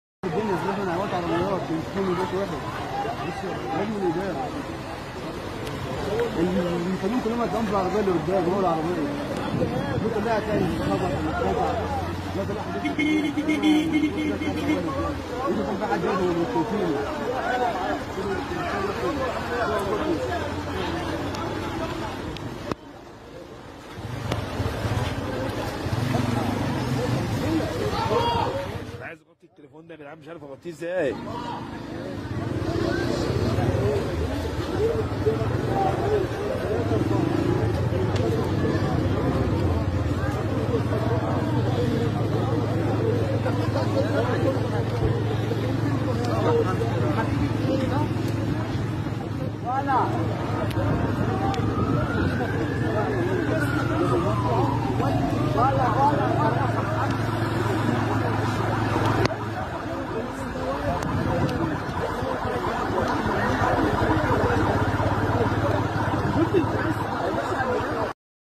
حادث تصادم قطارين بين رمسيس و امبابة بالقاهره بسبب الاحوال الجوية
السلامه عليكم ورحمه الله وبركاته
اهلا وسلا بكم فى قناة منوعات حول العالم
تعتمد قناة منوعات حول العالم عن كل ماهوة جديد حول العالم